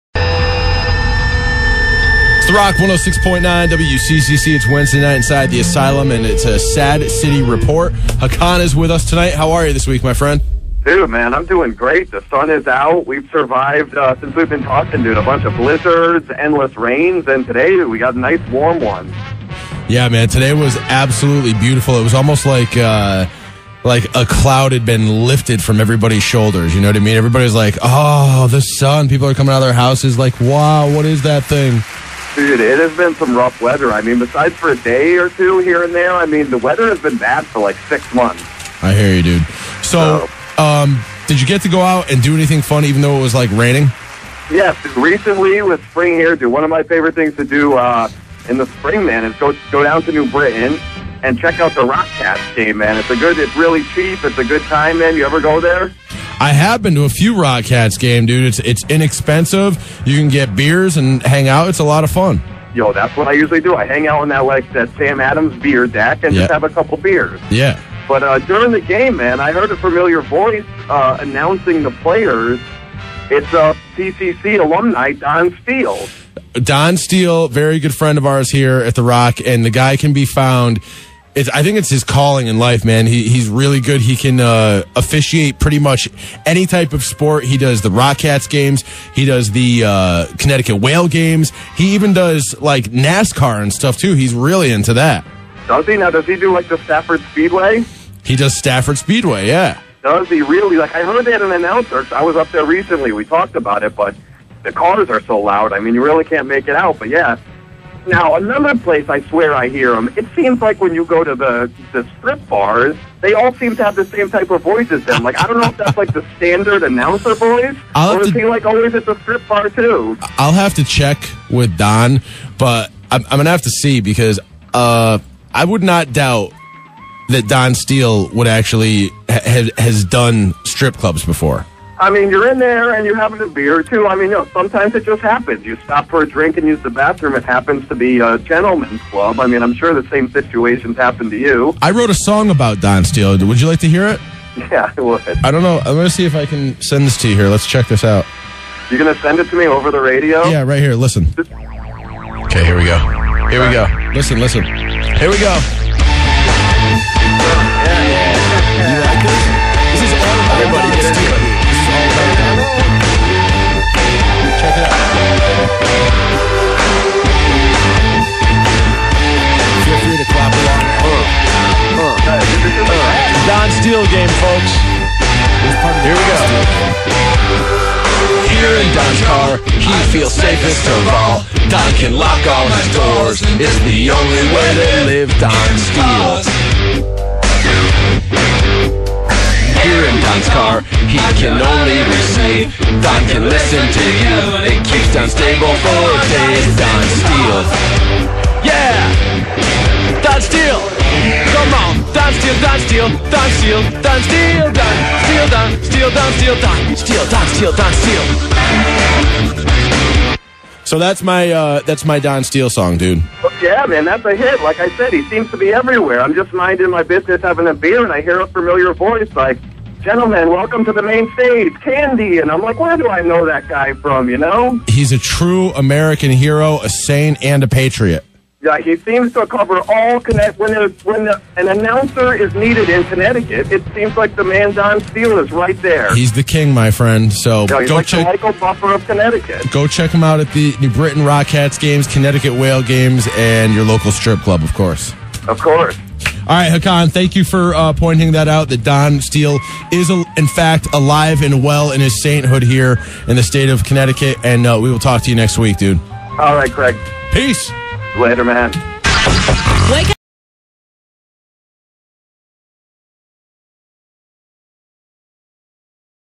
don steele.wmv
Sad City Hartford and Craig from WCCC discuss the legendary DON STEELE